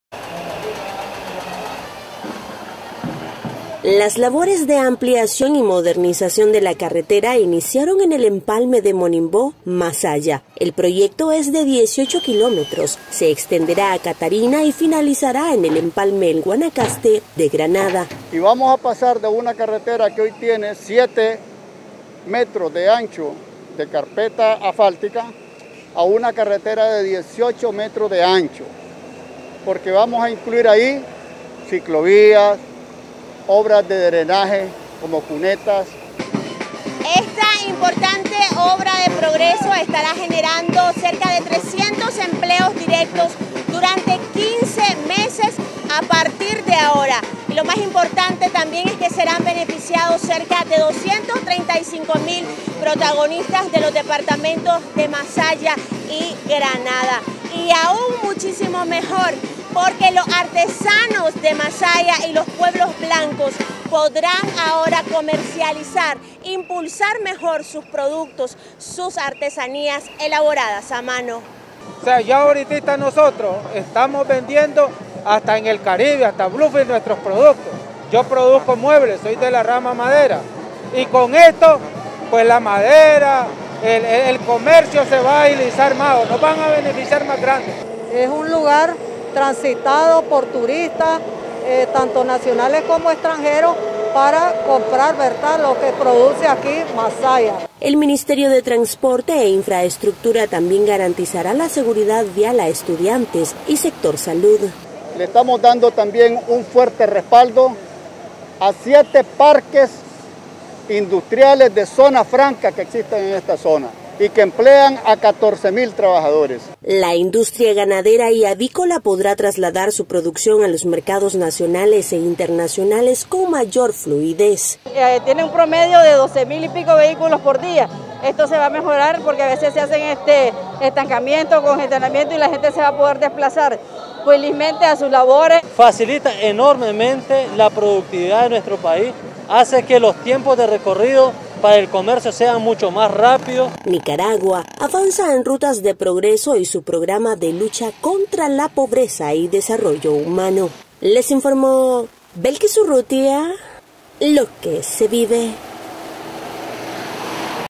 Inicia modernización de carretera Masaya - Catarina - Diriomo
Las labores de ampliación y mejoramiento de la carretera iniciaron en el empalme de Monimbó, Masaya. El proyecto es de 18 kilómetros, se extenderá a Catarina y finalizaran en el empalme El Guanacaste de Granada.